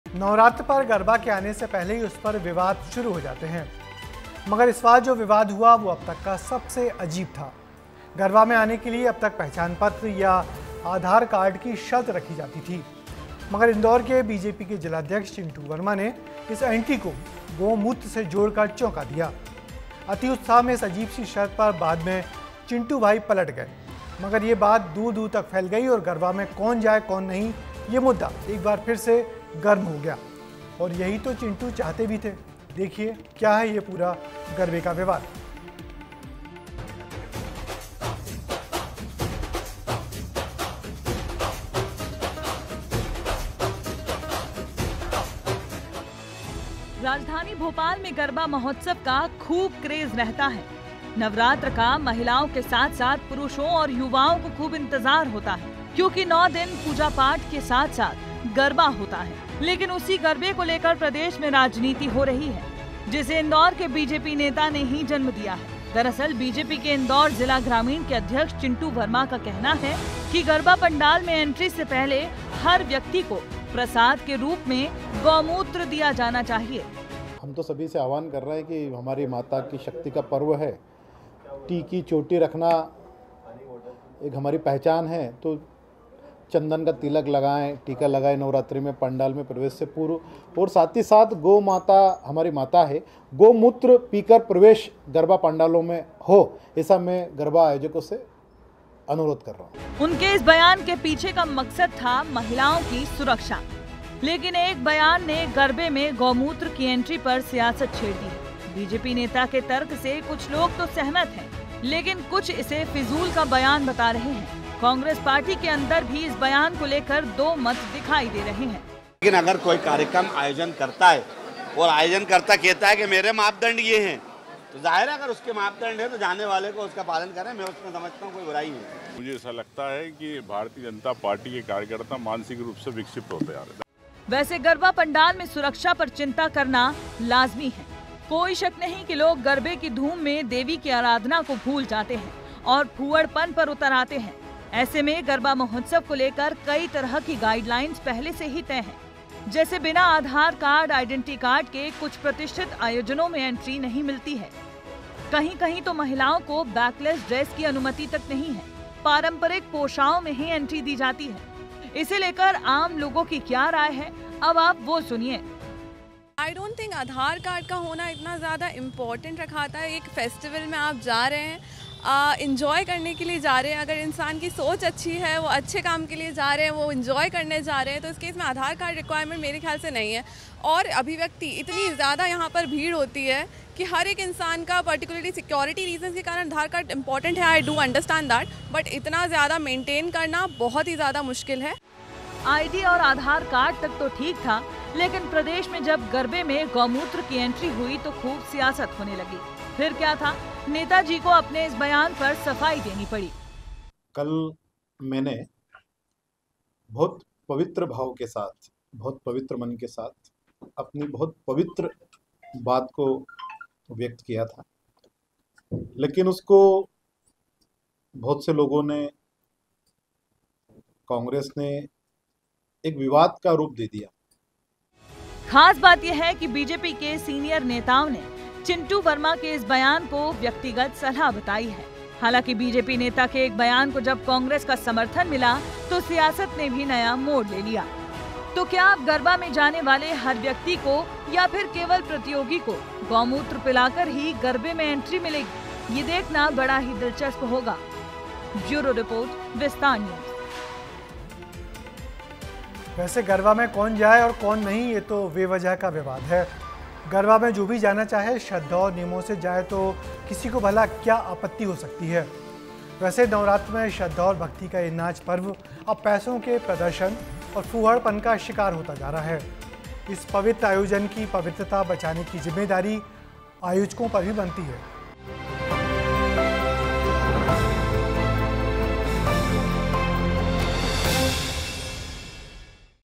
Navaratri में Garba करने के लिए क्या अब गौमूत्र पीना होगा ? क्या है गौमूत्र विवाद की पूरी सच्चाई ?
In this video, we delve into the recent controversy surrounding Garba celebrations, which has sparked significant debate even before the festivities begin. This year, the situation took an unexpected turn when Chintu Verma, the BJP district president of Indore, controversially linked entry to the event with the use of cow urine. Although he later retracted this unusual condition, the incident has already gained widespread attention, reigniting discussions about who is allowed to participate in Garba. Join us as we explore the full details of this peculiar Garba controversy.